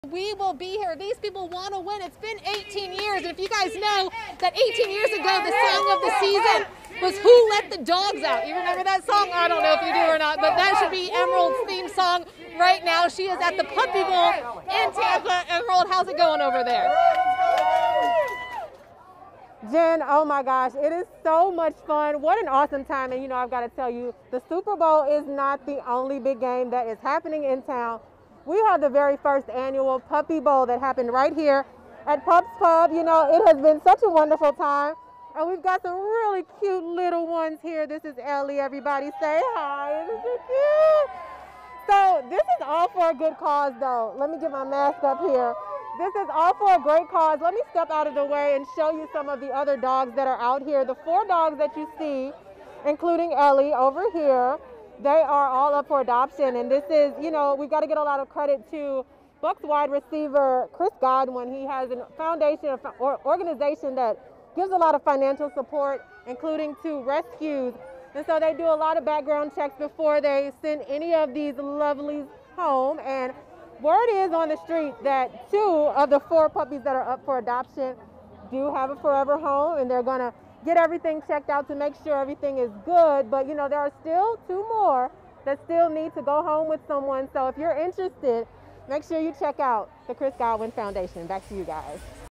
Dogs up for adoption at first annual Pups Pub Bowl in Tampa
As 10 Tampa Bay reporter Emerald Morrow tells us, the second-biggest event in town this weekend is surely the first annual Pups Pub Bowl at Pups Pub in Tampa.